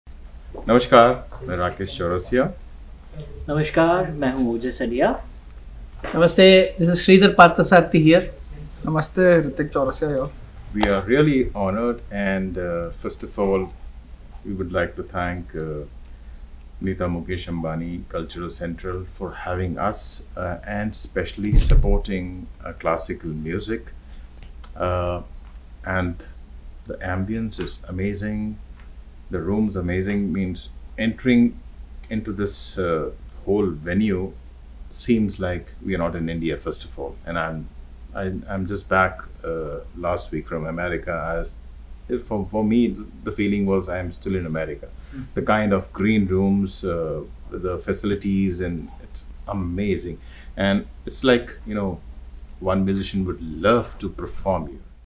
Team 'Hindustani Classical Flute Recital' Share Their Experience At The Studio Theatre
Yesterday at the NitaMukeshAmbaniCulturalCentre, we saw 'Hindustani Classical Flute Recital' - a surreal evening by veteran flautist Rakesh Chaurasia who brought his mesmerising tunes and mellifluous renditions to the stage. Accompanied by Ojas Adhiya on tabla and Sridar Parathasarthy on mridangam, the Hindustani Classical music exponent’s performance showcased his ability to blend seamlessly while preserving the authenticity of the music. Watch the artist share his experience at TheStudioTheatre.